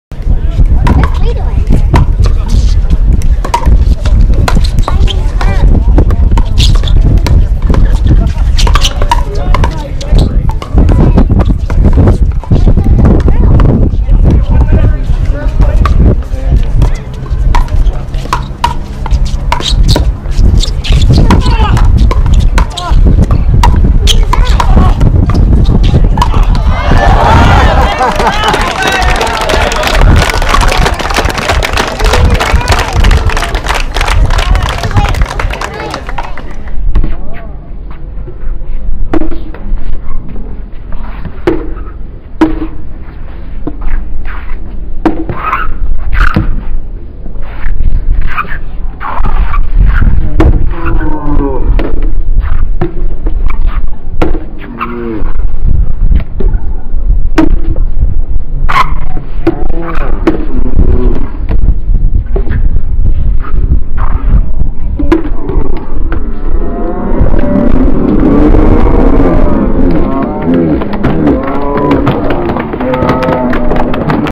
2021 Pickleball Canada Nationals: Allez! [Nice Rally + Defence]
August 13, 2021. Pickleball Canada Nationals, Red Deer, Alberta.
Bradwell/Rich vs Lambert/Fajardo.
Bronze Medal Match, Mixed Doubles Open [17-49 age].